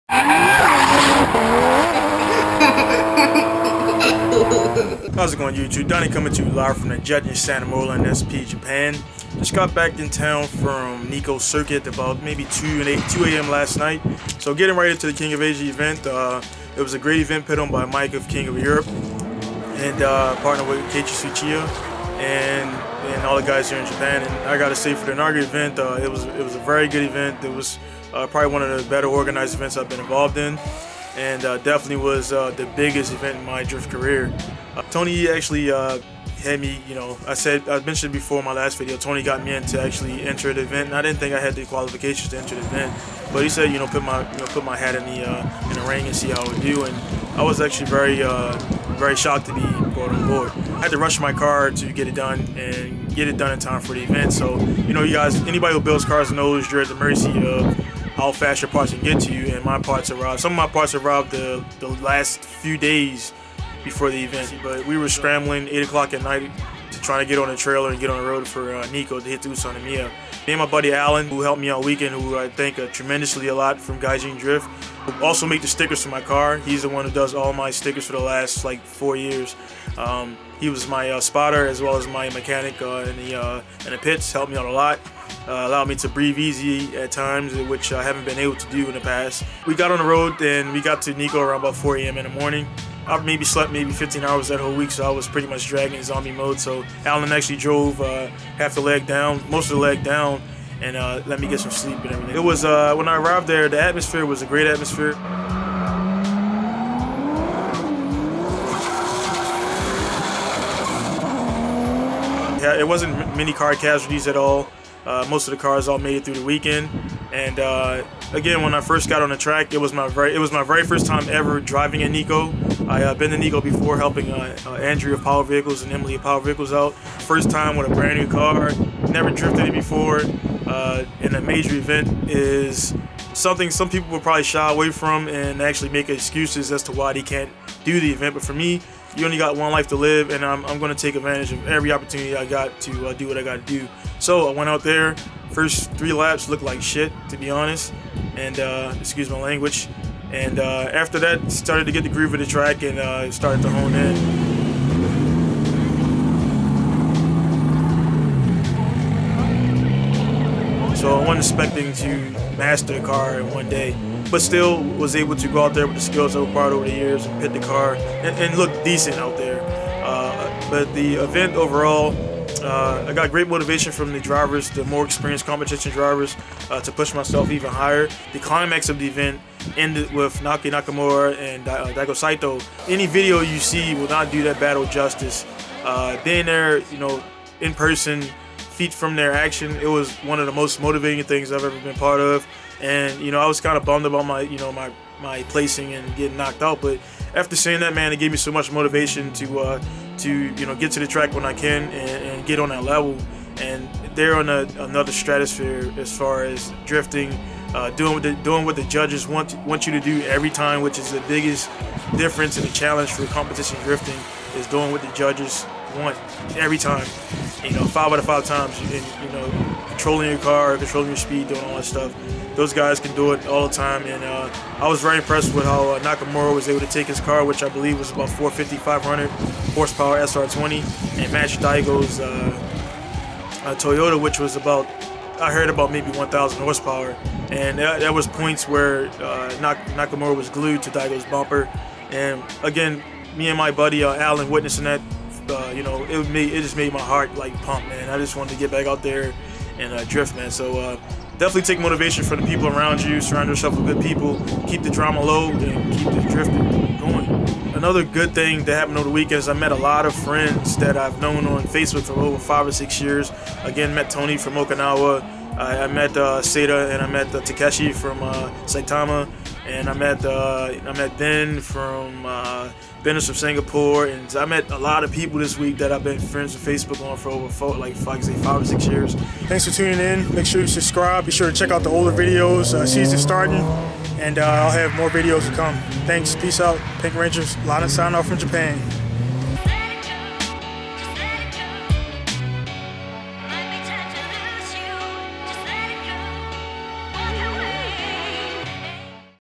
Pink Ranger Drift: King of Asia Nikko Recap
Hello everyone just winding down from this weekend at King of Asia my first pro level drift event. Was a great event and only the start of King of Asia.